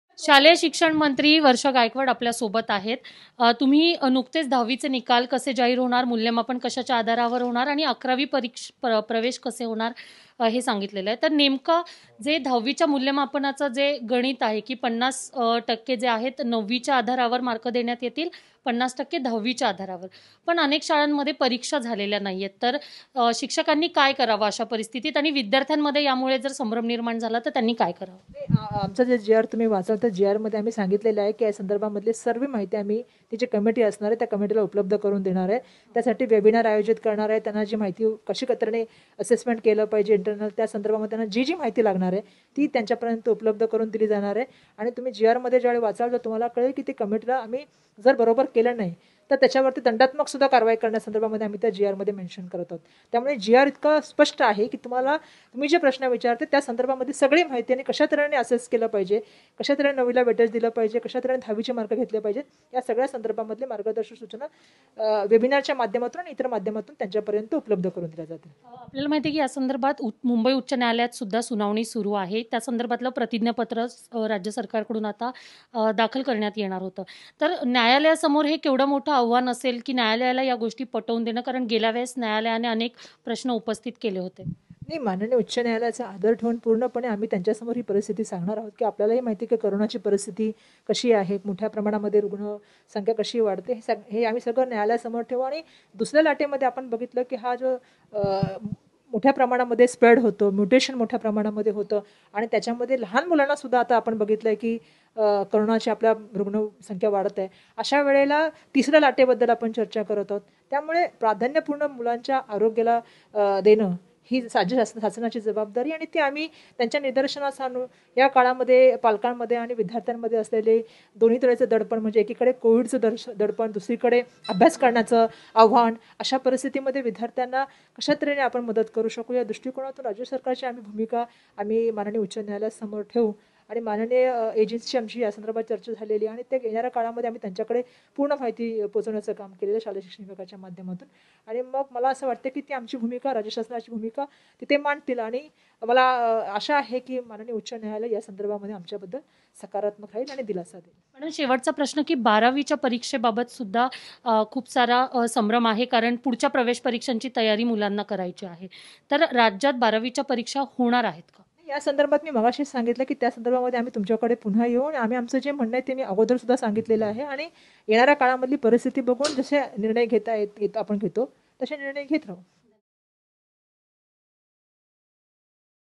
HSC Exam board timetable, SSC वर Varsha Gaikwad यांनी Uddhav Thackeray सरकारची भूमिका केली स्पष्ट
कोरोनाच्या पार्श्वभूमीवर 2020-21 या शैक्षणिक वर्षाची दहावीच्या परीक्ष रद्द करून, सर्व विद्यार्थ्यांना अंतर्गत मूल्यमापनाच्या आधारे सरसकट उत्तीर्ण करण्याबाबत परवानगी महाराष्ट्राच्या मंत्रिमंडळाने दिली आहे. शालेय शिक्षणमंत्री वर्षा गायकवाड यांनी ही माहिती दिली. दहावीच्या विद्यार्थ्यांच्या निकालासाठी 100 गुणांचं मूल्यमापन करण्यात येईल. नववी आणि दहावीचे शाळाअंतर्गत मूल्यमापन करण्यात येईल. त्यानंतर जून अखेरपर्यंत दहावीचा निकाल जाहीर करण्यात येईल. याविषयी बीबीसी मराठीच्या प्रतिनिधी दिपाली जगताप यांनी शिक्षणमंत्री वर्षा गायकवाड यांच्याशी संवाद साधला.